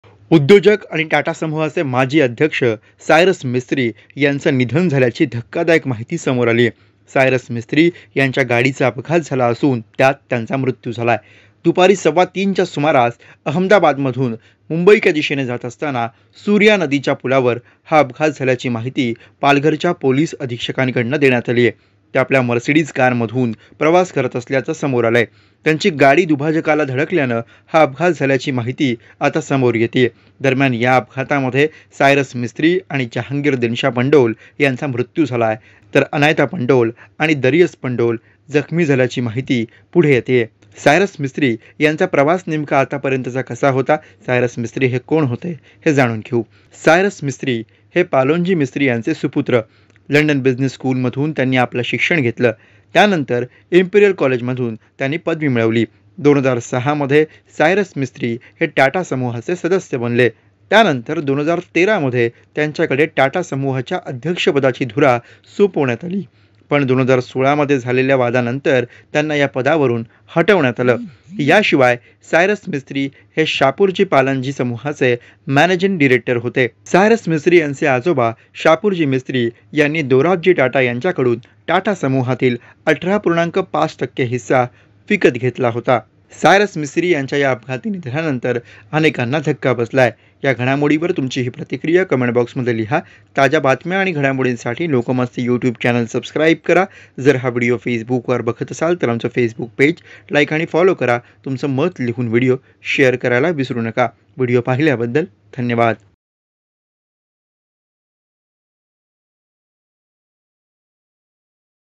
सायरस मिस्त्री यांचं अपघाती निधन, अपघात नेमका कसा झाला? How exactly Cyrus Mistry Accident happen?
उद्योजक आणि टाटा समूहाचे माजी अध्यक्ष सायरस मिस्त्री यांचं निधन झाल्याची धक्कादायक माहिती समोर आली आहे. सायरस मिस्त्री यांच्या गाडीचा अपघात झाला असून त्यात त्यांचा मृत्यू झाला. दुपारी सव्वातीनच्या सुमारास अहमदाबात येथून मुंबईच्या दिशेने जात असताना सूर्या नदीच्या  पूलावर हा अपघात झाल्याची माहिती पालघरच्या पोलीस अधीक्षकांडून देण्यात आली. ते आपल्या मर्सिडिज कारमधून प्रवास करत असल्याचं समोर आली आहे. त्यांची गाडी दुभाजकाला धडकल्यानं हा अपघात झाल्याची माहिती समोर येतेय.,, (Guruprasad VO)